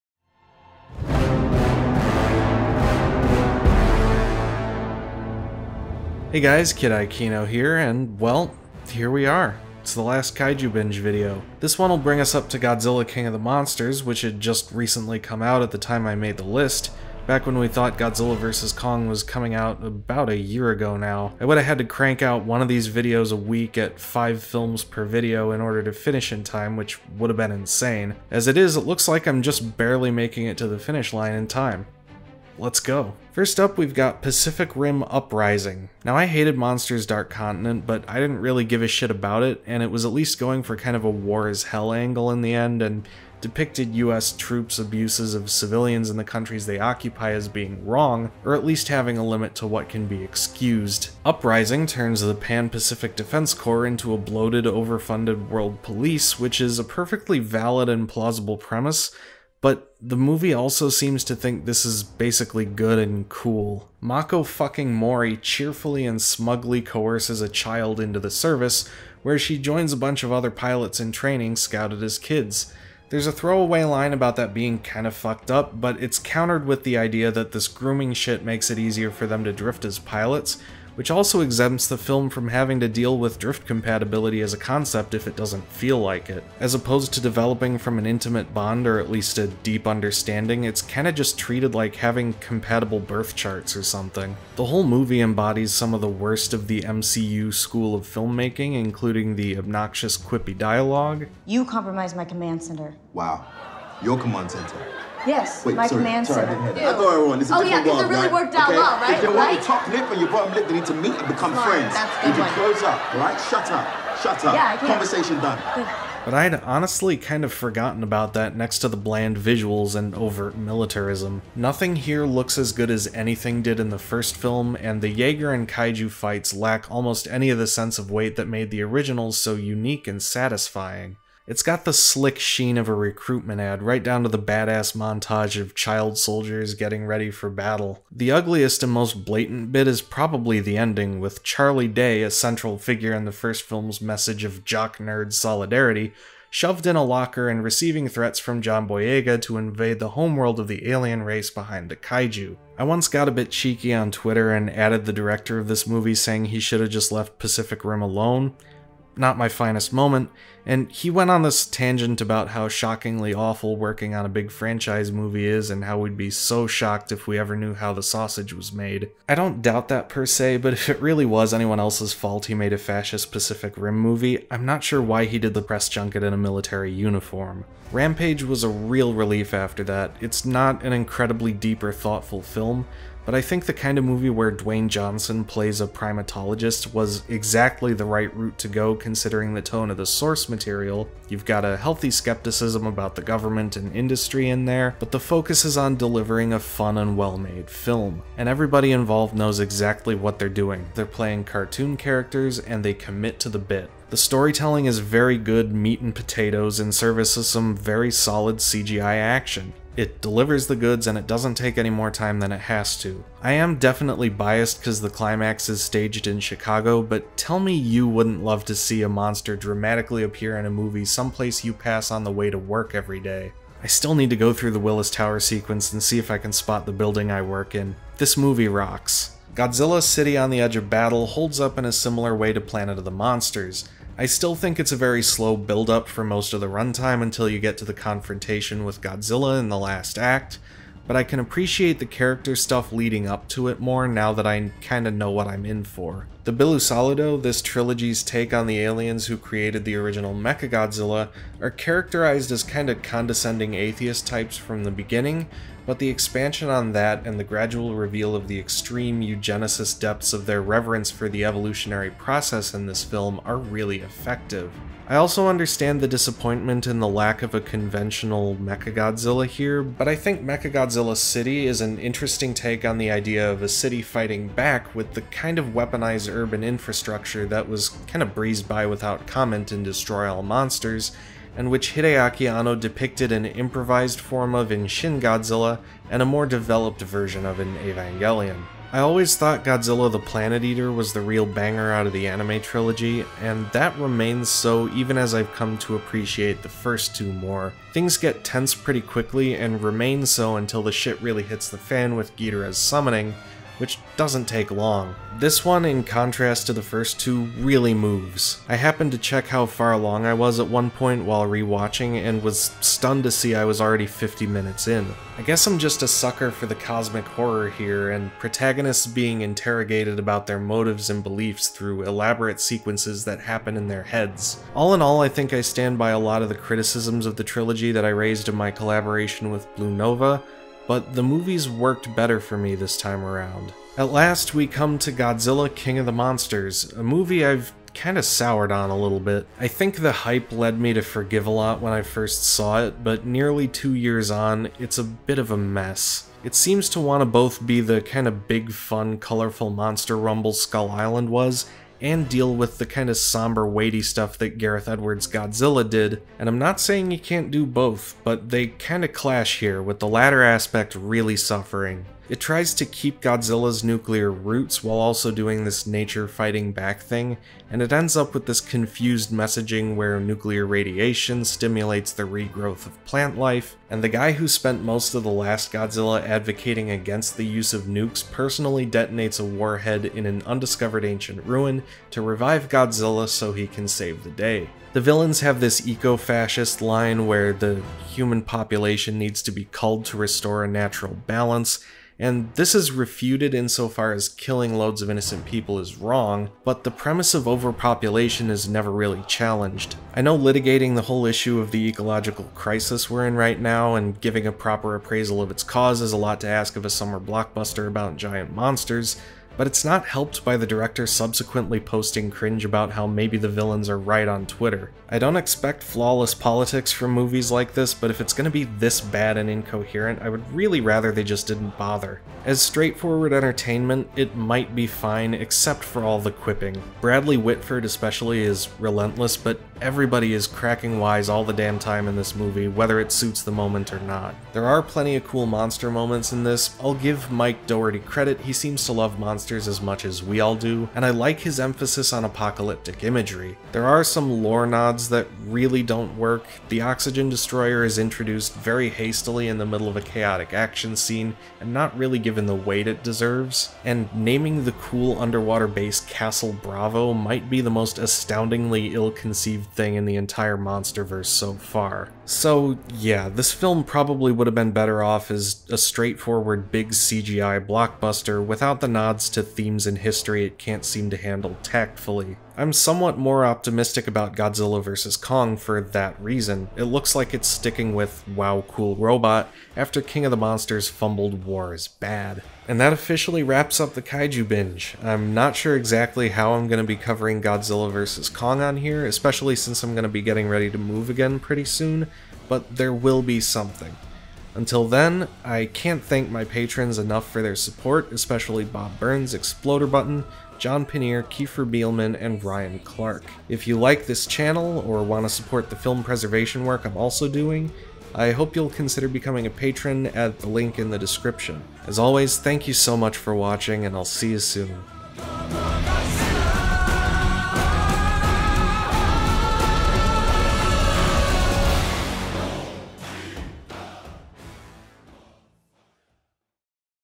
Ultimate Kaiju Binge #20: Royalty, religion, and the Rock
The final episode brings us to King of the Monsters, just before the pandemic. People gloried in the peaceful day, unaware of the coming tragedy!

This is a critical/educational work and constitutes fair use of copyrighted material under US law.